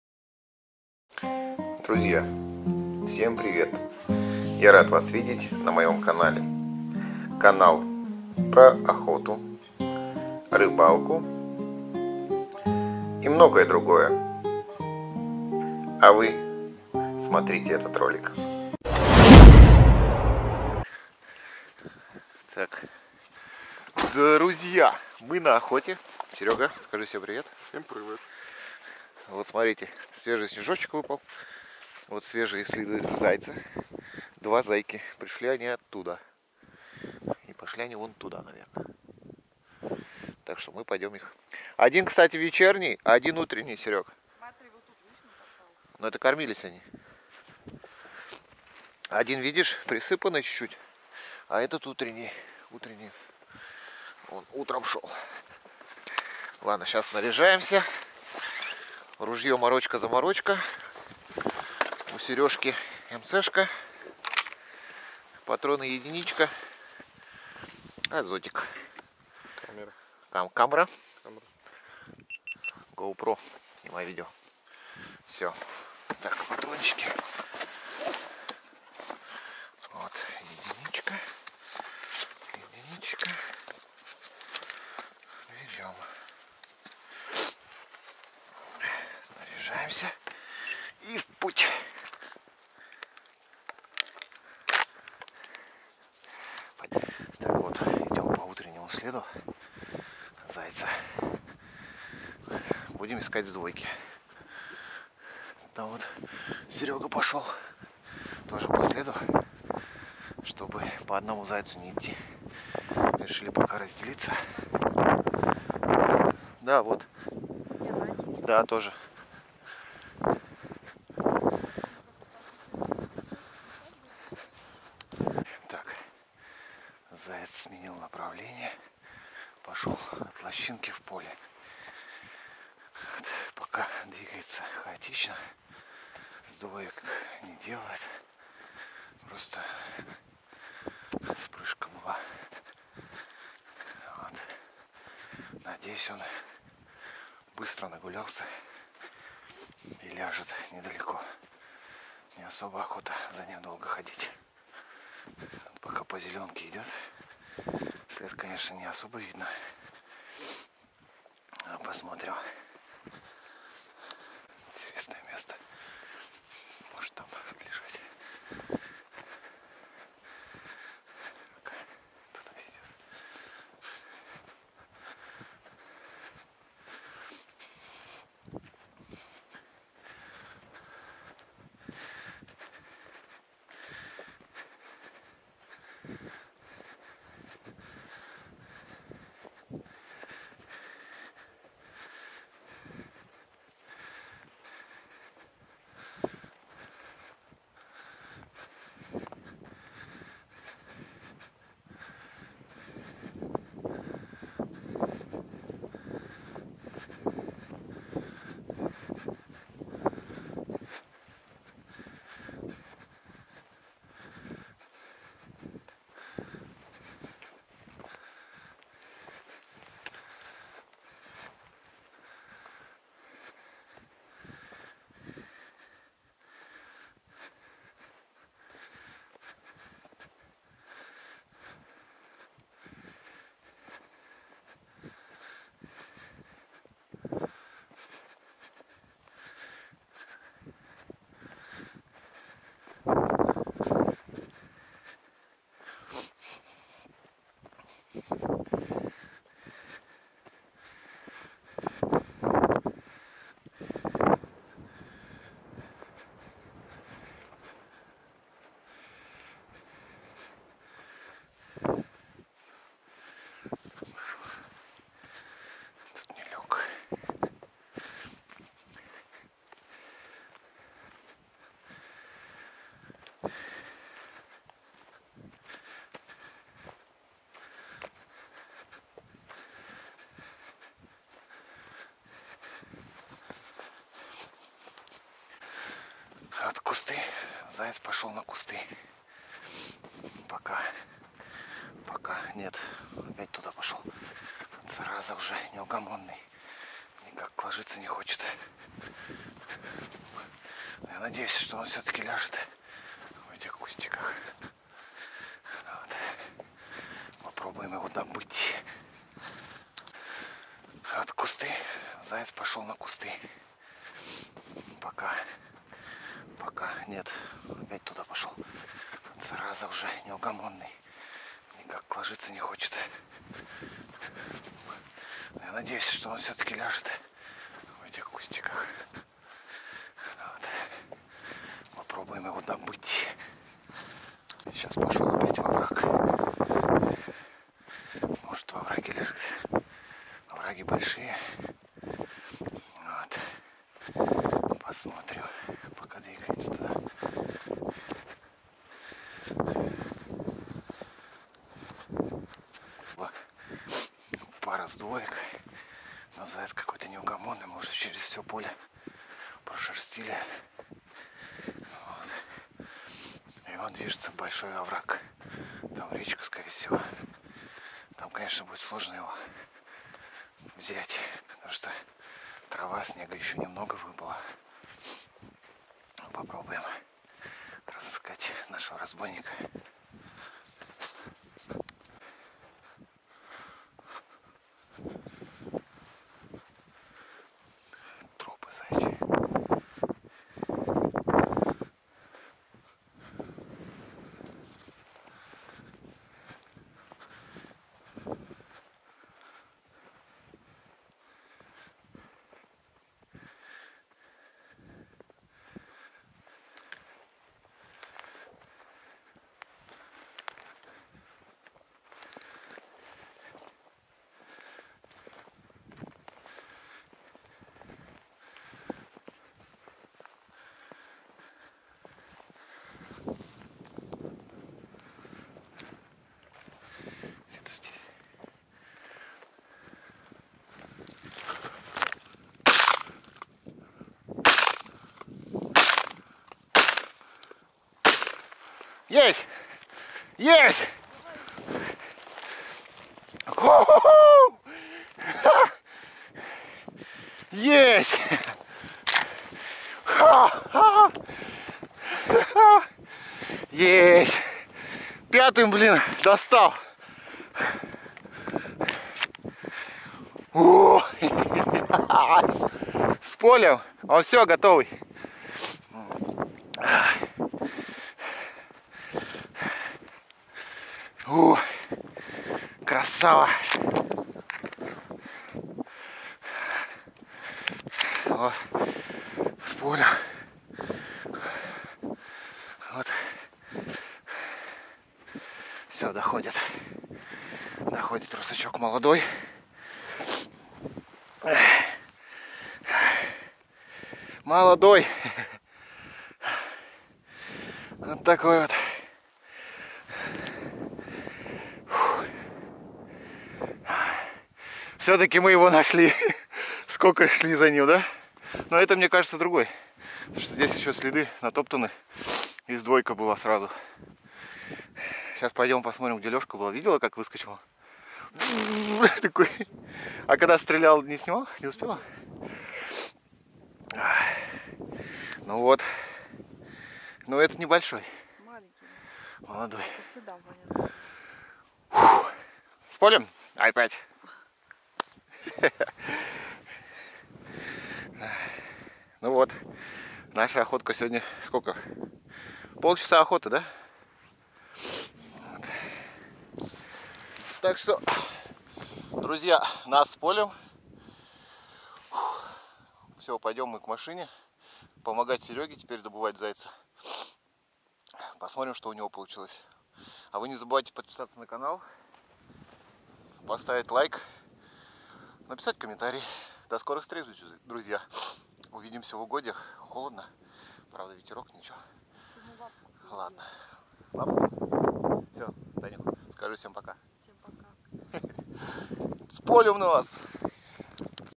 ОХОТА НА ЗАЙЦА , ДОБЫЛ УШАСТОГО .
НА ВИДЕО ВЕСЬ ПРОЦЕСС ОХОТЫ НА ЗАЙЦА .